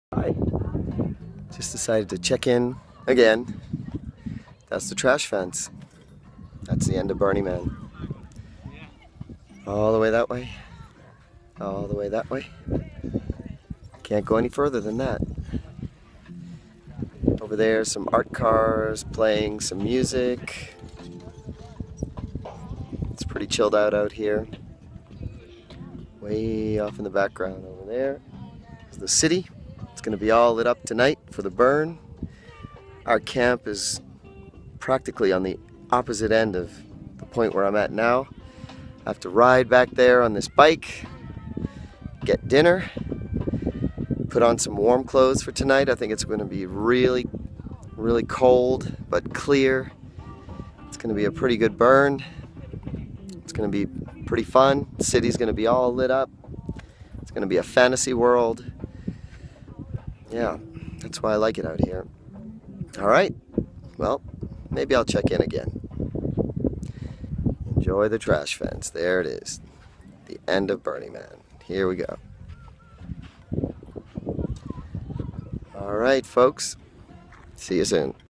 Burning Man 2015 - Check in at the Trash Fence
Took a ride to the Trash Fence, a very solemn place of deep contemplation.